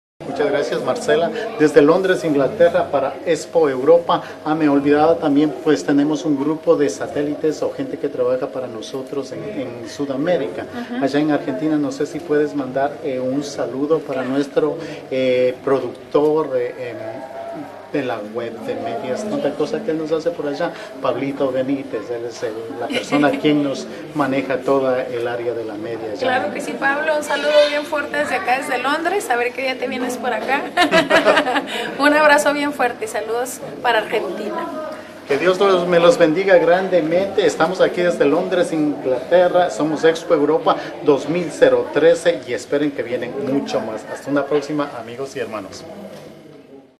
Saludos de Marcela Gandara
Marcela Gandara en su visita a Londres - Inglaterra envió un saludo al fundador de radioelaltisimo.com y Visión Proactiva Group Internacional